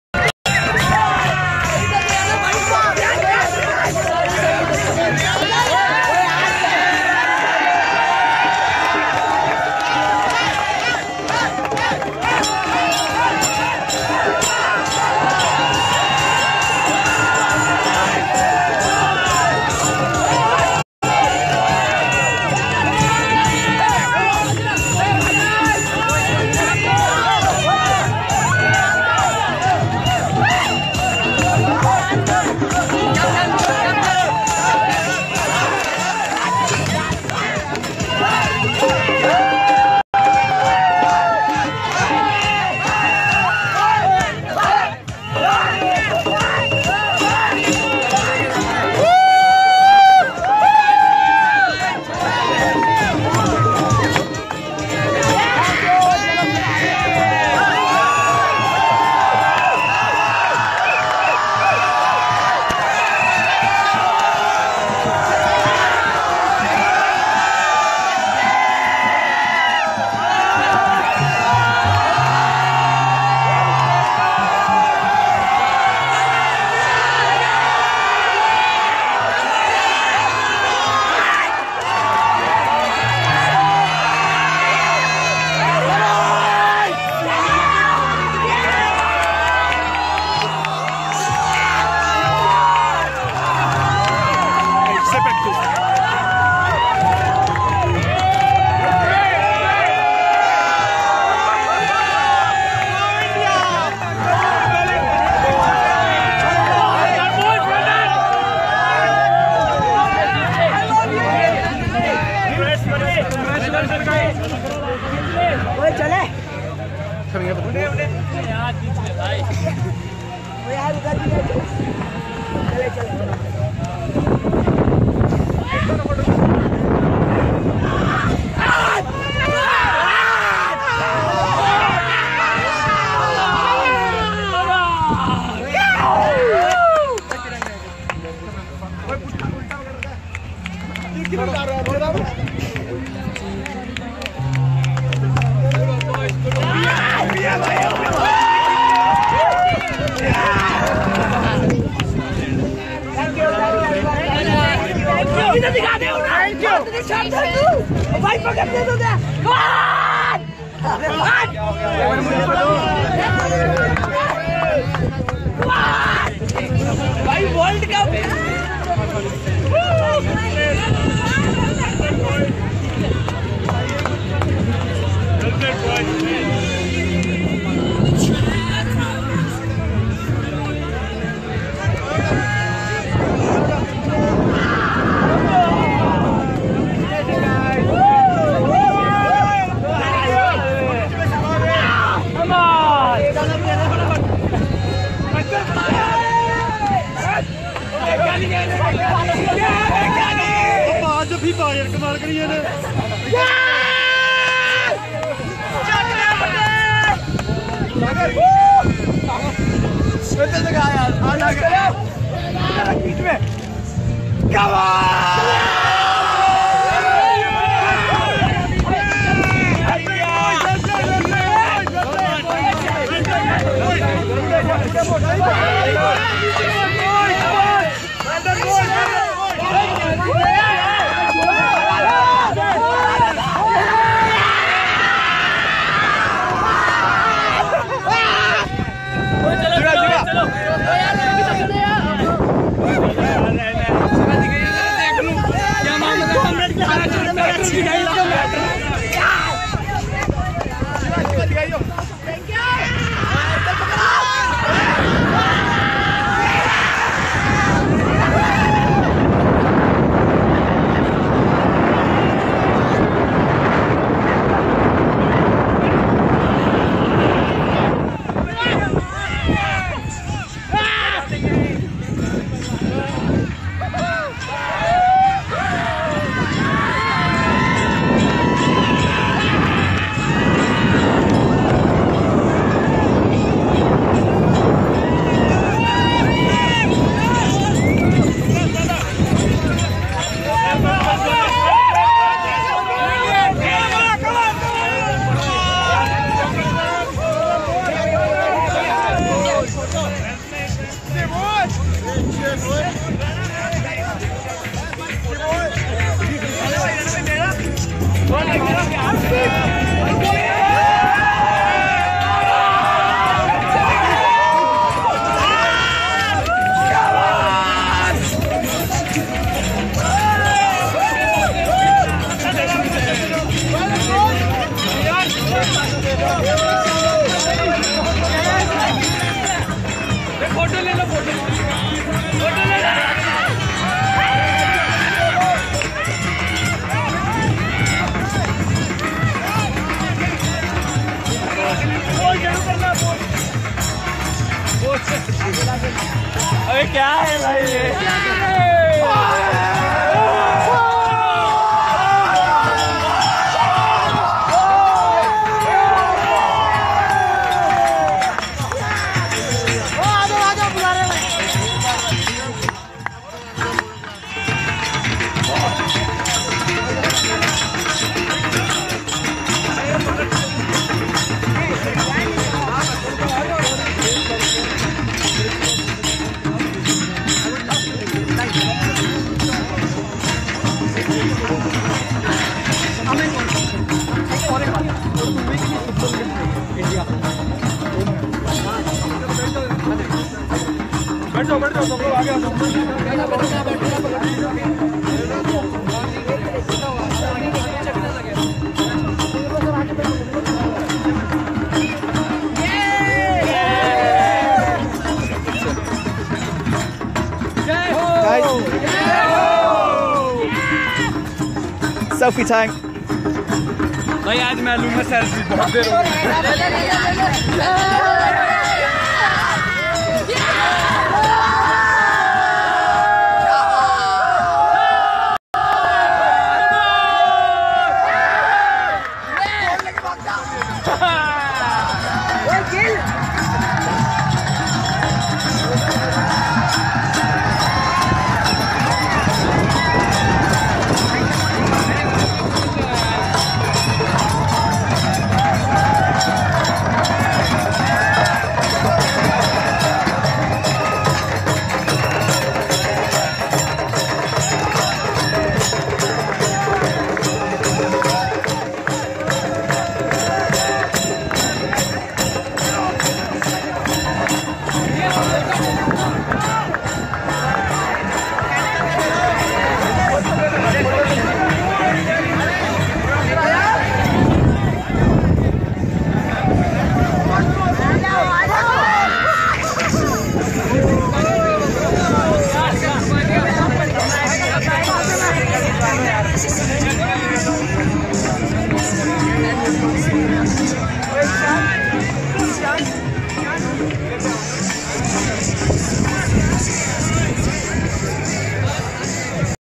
Indian Team Celebration After Winning U -19 World Cup Match Against Australia
Indian team Celebration after Winning Under 19 world coup against Australia